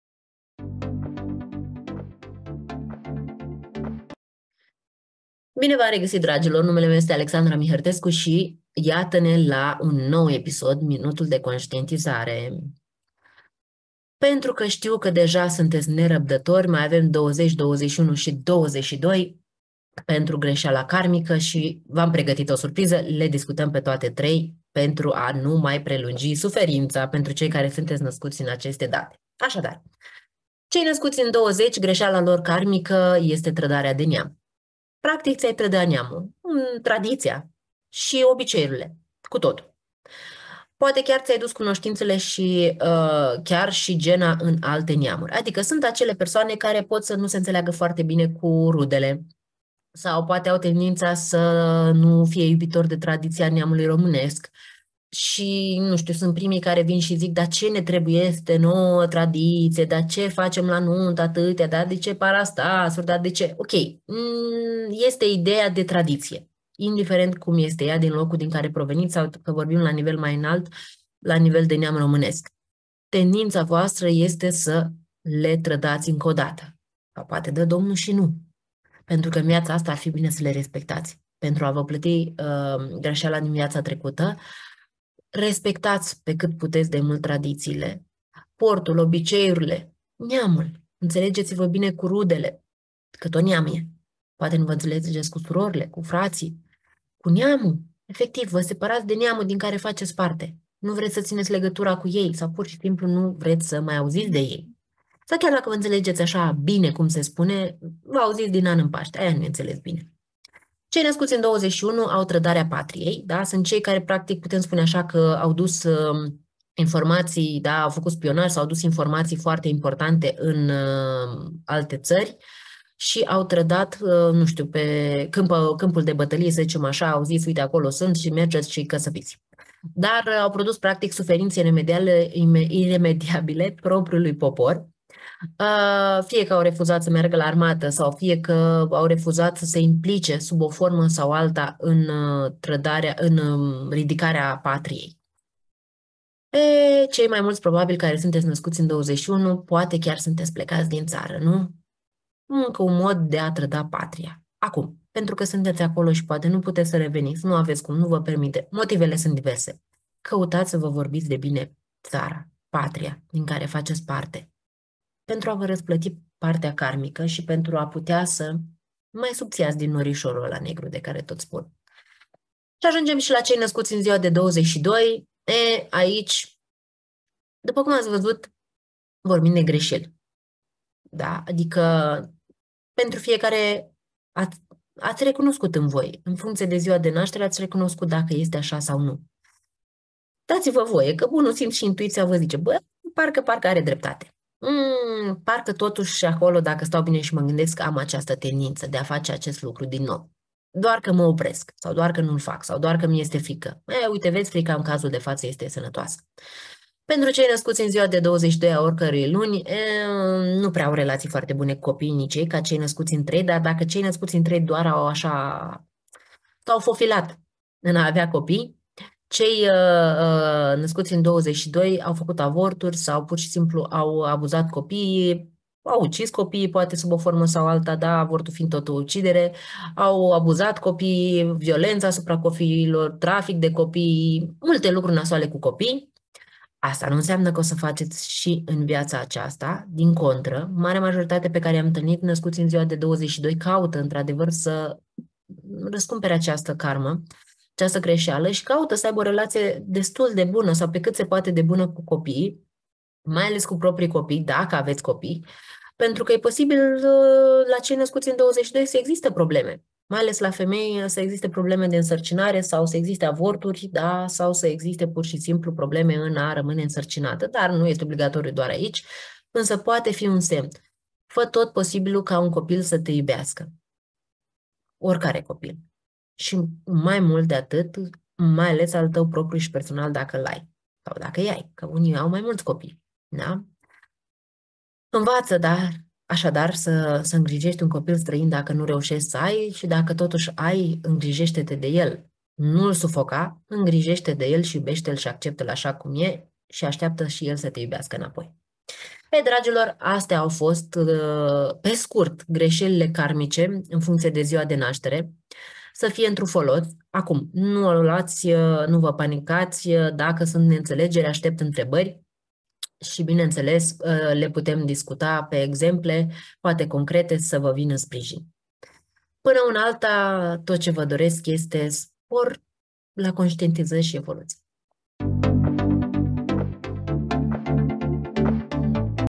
Minutul de conștientizare - Greșeala karmică a celor născuți în ziua de 20, 21 si 22
Am pregătit o surpriză astăzi pentru tine! Te las să descoperi în filmul de mai jos care este greșeala celor născuți în 𝐳𝐢𝐥𝐞𝐥𝐞 𝐝𝐞 𝟐𝟎, 𝟐𝟏 𝐬̦𝐢 𝟐𝟐! Și asa a luat final călătoria noastră în sinteza greșelilor karmice! Vă reamintesc că aici am surprins ideil pe scurt. Cei ce doresc să aprofundeze acest domeniu poate apela la cursul maestrului Anatol Basarab - numerologie karmică!    Ai vizionat celelalte episoade? O poți face direct aici
c/AlexaEvent333/